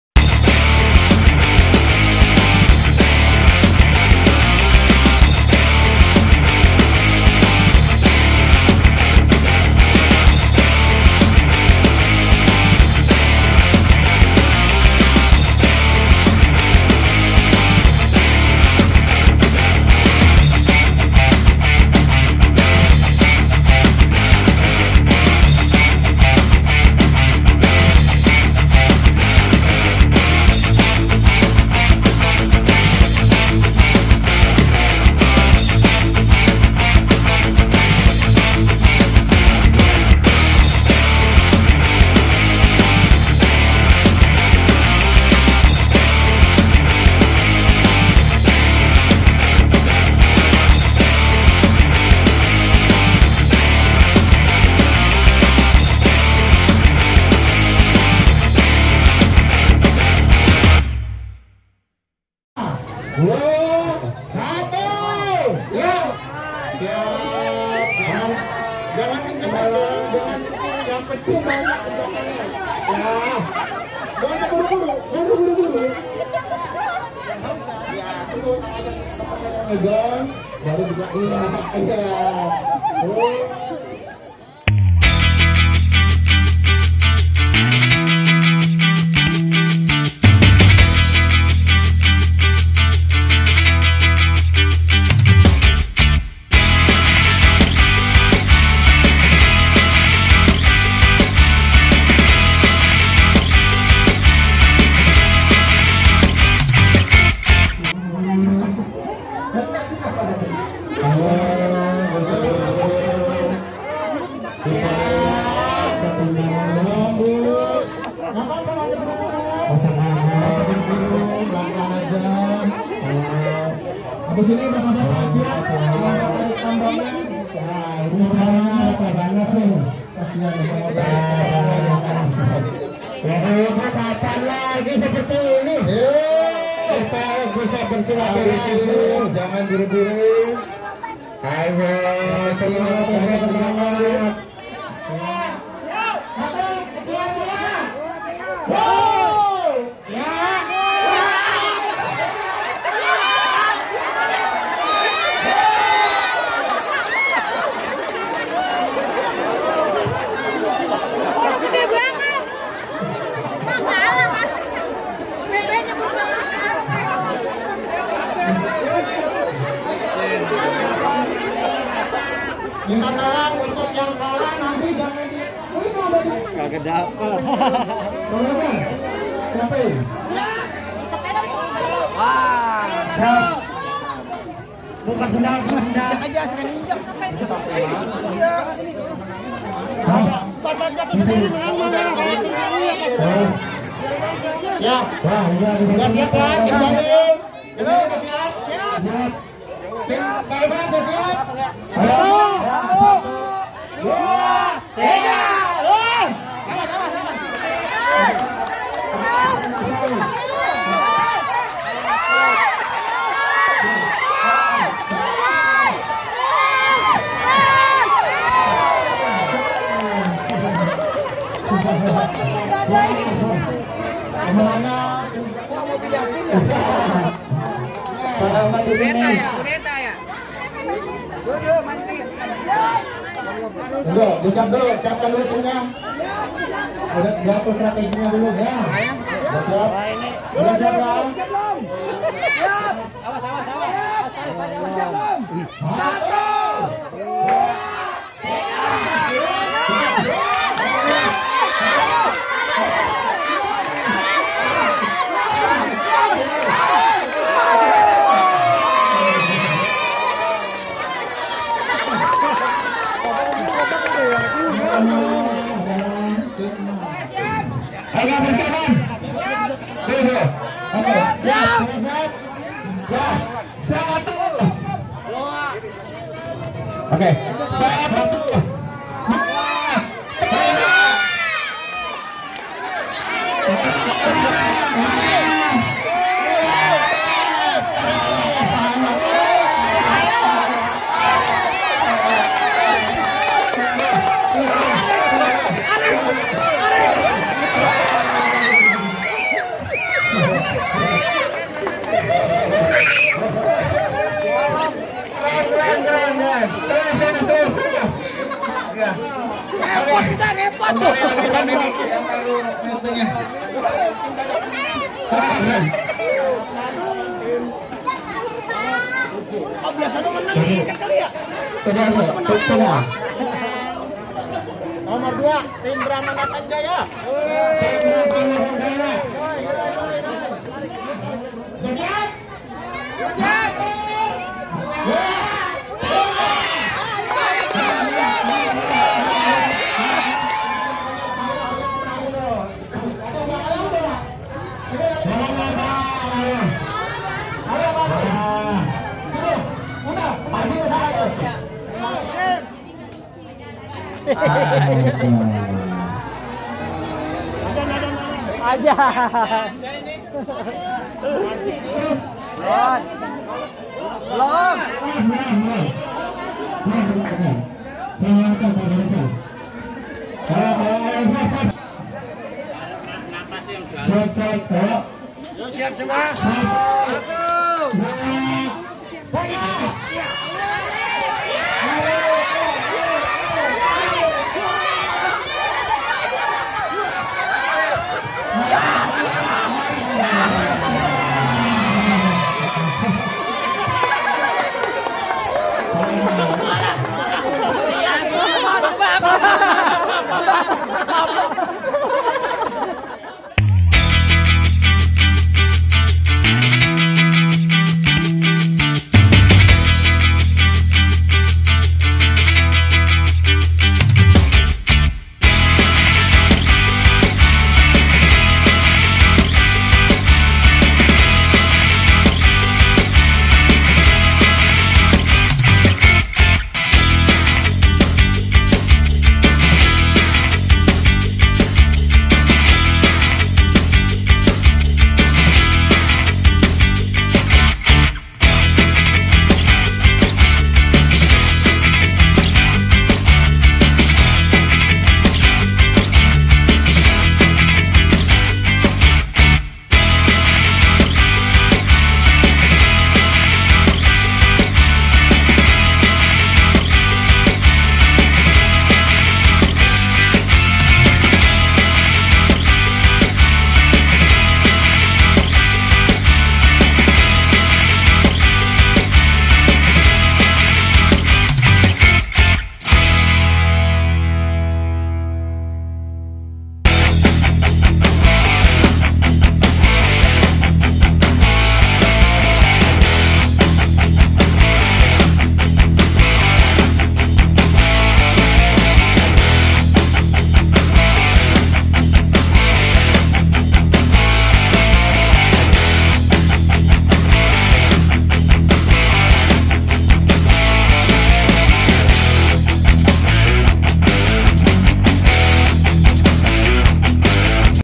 Lomba agustusan lucu dan seru | RT 05
video kali ini saya mendokumentasikan keseruan lomba agustusan warga Rt 05

👉Dukung terus channel saya
👉SUBS LIKE AND COMENT